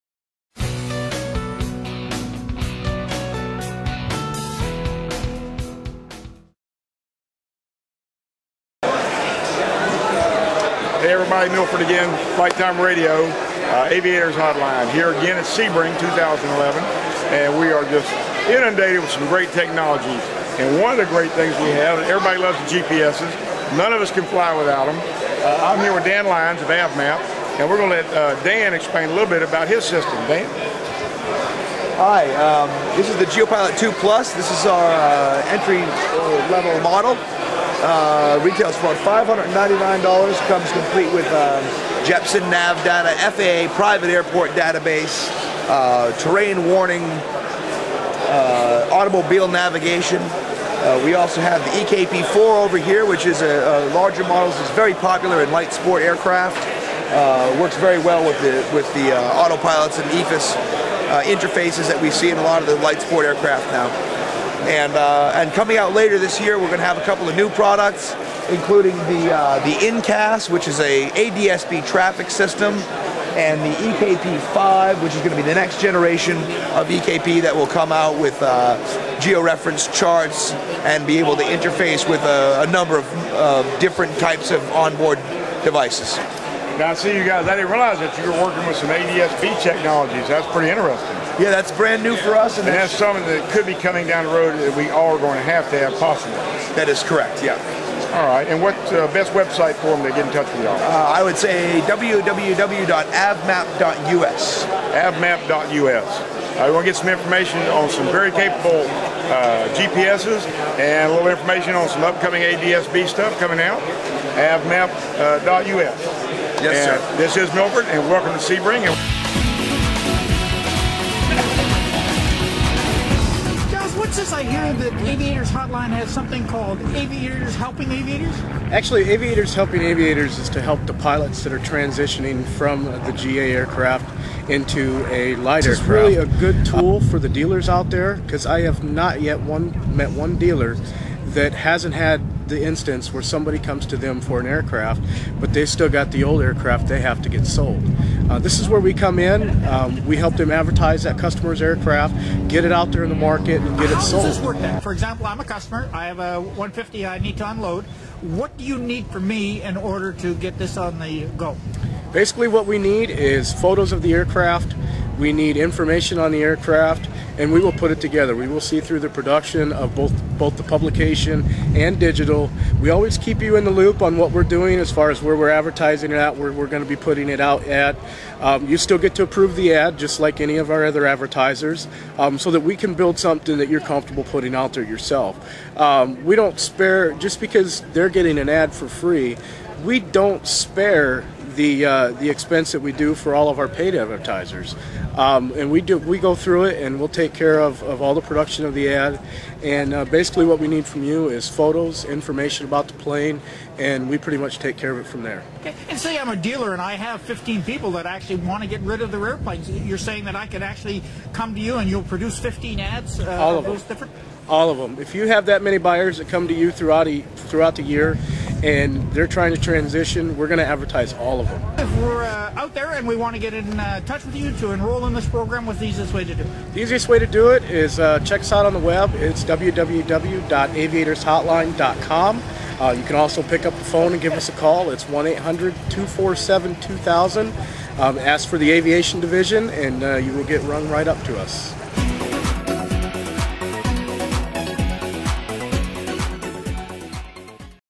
AVMAP - Aviation Weather and Real Time Aviation Traffic
We talk to the people from AVMAP about their latest products at the U.S. Sport Aviation Expo

PCAS XRX compatible software for a new level of awareness!

You can now display real time traffic info from the Zaon XRX on your EKP-IV, EKP-IV PRO or GeoPilot II+. The 3 closest traffic threats will be displayed right on the screen!

Traffic depiction is color coded for instant threat level recognition, and values are given for altitude and whether the aircraft is ascending or descending